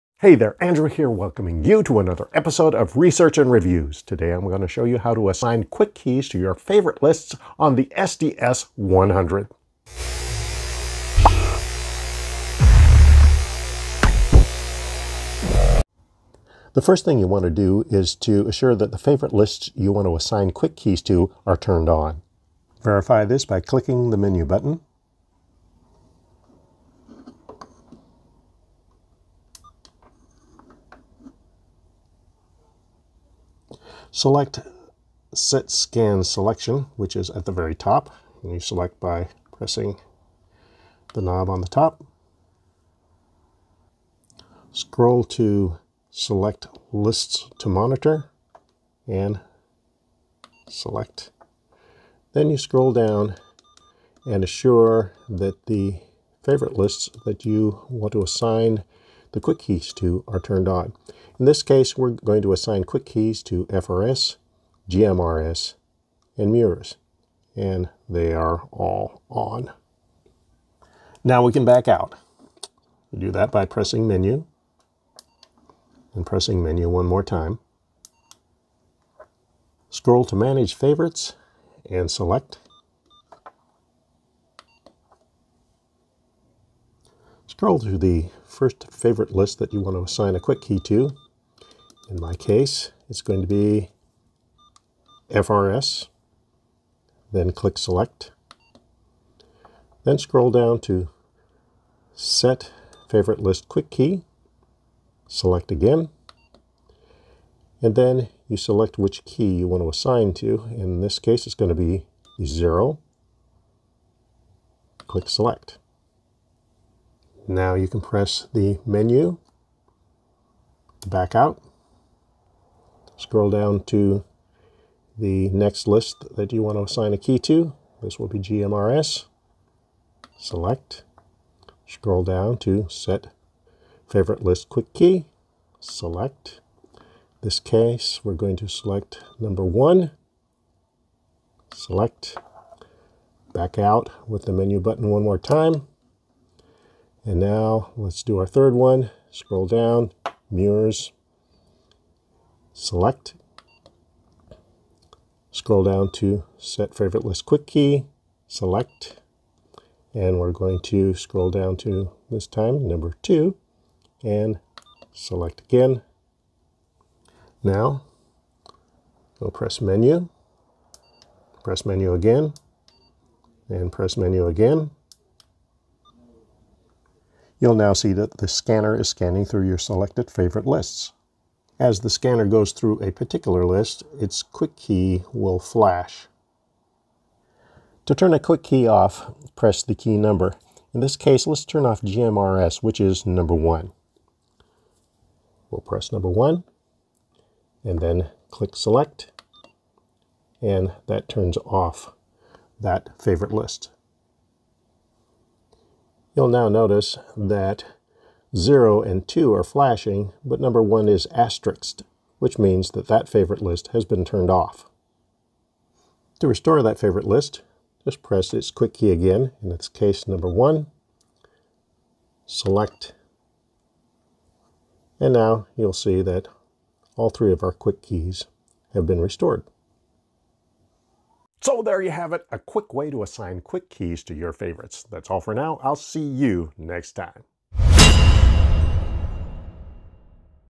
How to set up Quick Keys on the SDS100!
A tutorial on setting up quick keys on the SDS100.